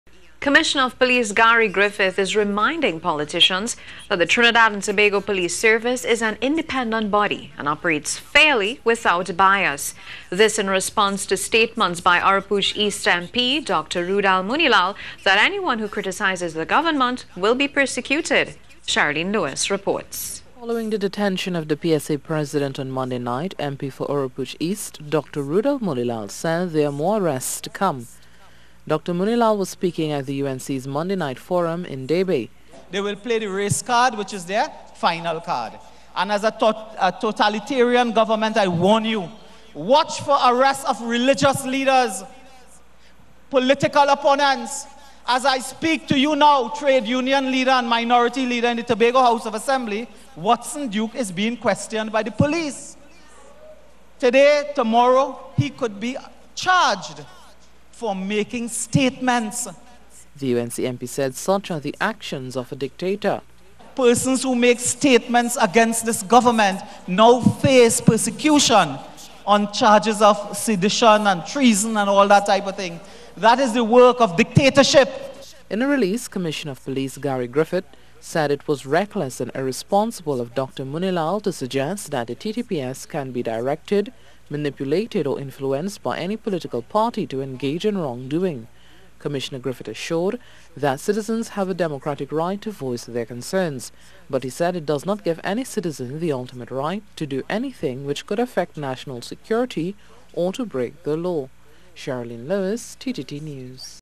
Police Commissioner Responds To Statements Made By Roodal Moonilal
Commissioner of Police Gary Griffith is reminding Politicians that the Trinidad and Tobago Police Service (TTPS) is an independent body, and operates fairly without bias.

This in response to statements by Oropouche East MP Dr. Roodal Moonilal, that anyone who criticises the government, will be persecuted.

Cherrylene Lewis reports.